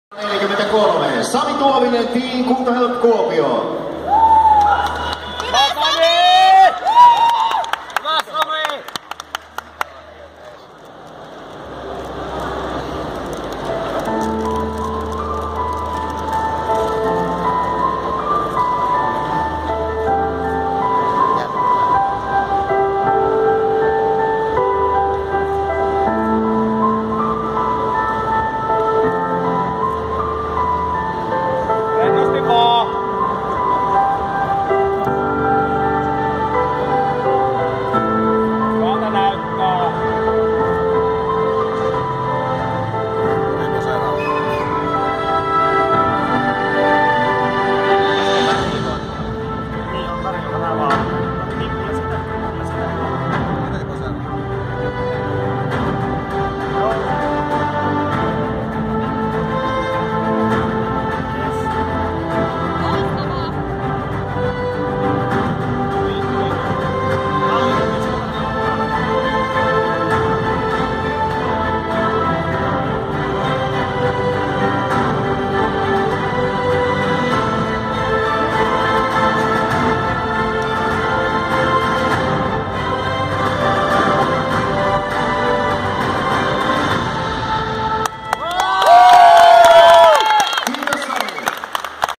Sami Tuovinen posing at the NFE 2016
5. place finisher in Classic BodyBuilding -178 cm class, at Finnish Nationals (NFE 2016)